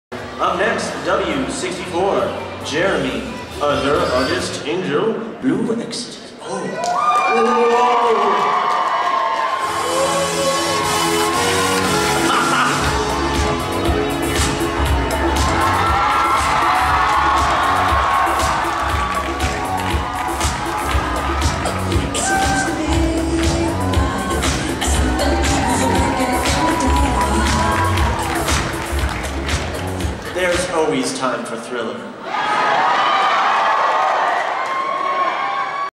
Anime Central 2012 - W34 Blue Exorcist
Video of Walk-on #34 - Blue Exorcist . For all videos, please visit our convention